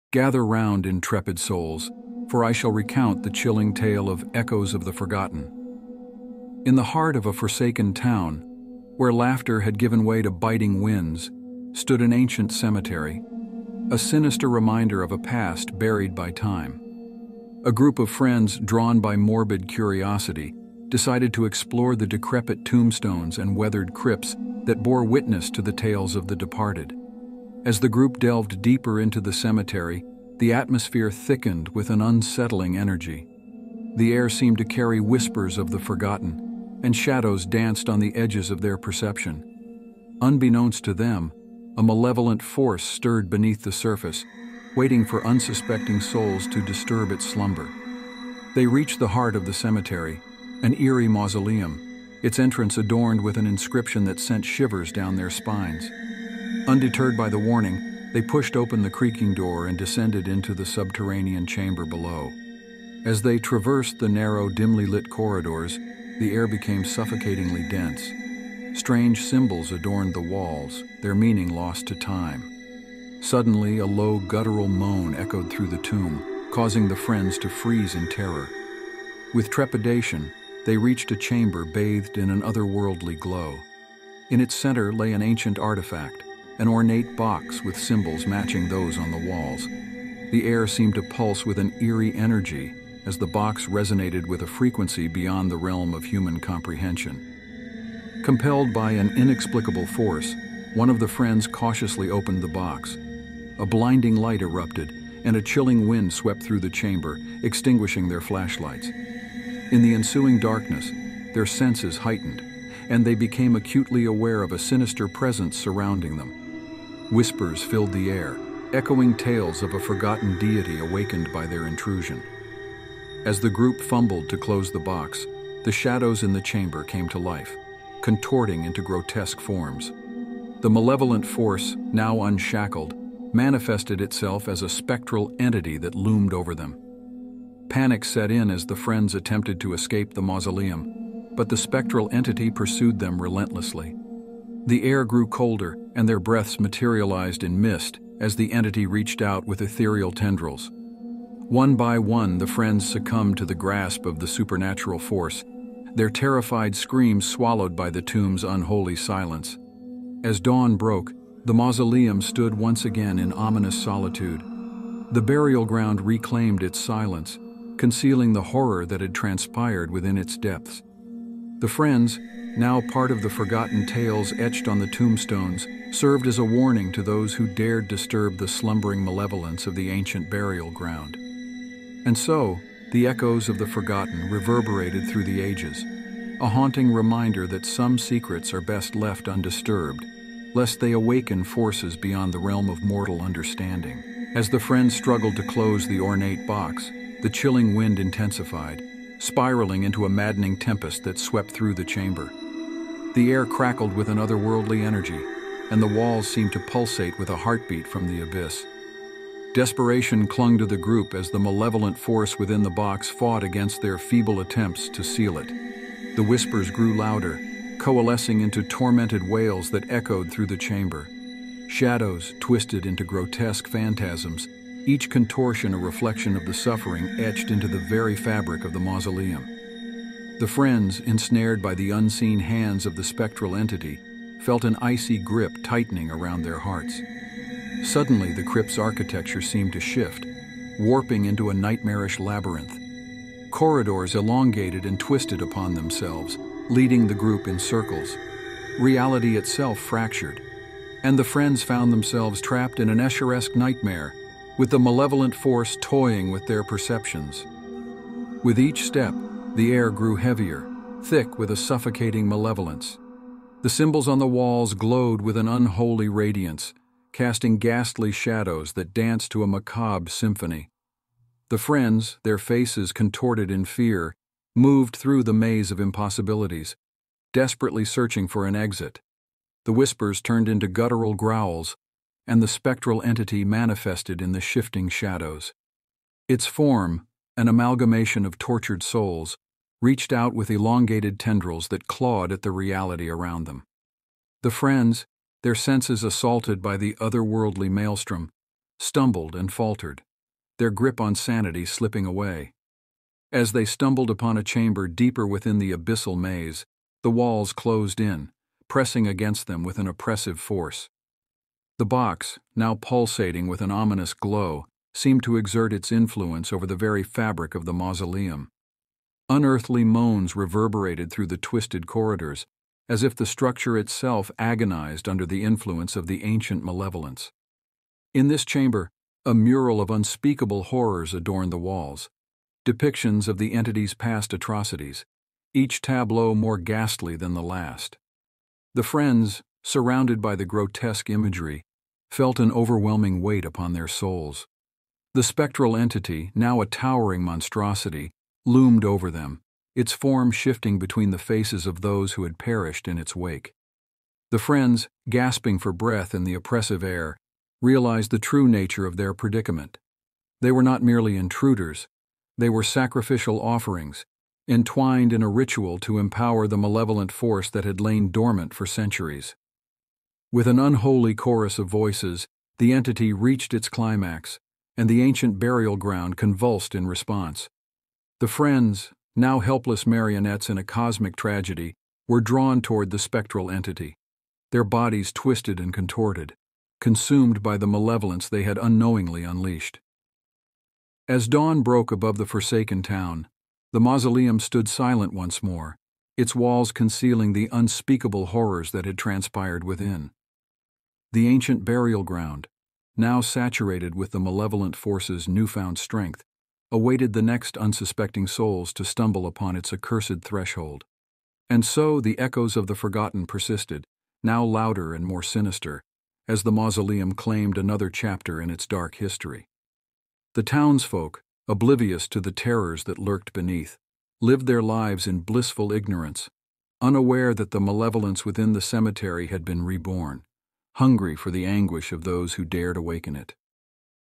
The Awakening of Forgotten Shadows,"The Abyssal Reckoning"
"The Abyssal Reckoning"

In the chilling tale of "The Abyssal Reckoning," a group of friends, driven by morbid curiosity, stumbles upon an ancient burial ground harboring an ominous secret. As they unwittingly open an ornate box within a decaying mausoleum, they unleash a malevolent force that distorts reality, transforming the burial ground into an Escheresque nightmare. The friends become ensnared in a labyrinth of impossibilities, pursued by a spectral entity fueled by the tormented souls of the past. In a chamber adorned with macabre murals, they discover the true nature of their predicament—a sacrificial ritual to empower an ancient evil. As the ancient burial ground absorbs the newfound malevolence, it hungers for more unsuspecting souls, leaving behind a legacy of horror and an echoing reminder that some secrets are best left undisturbed.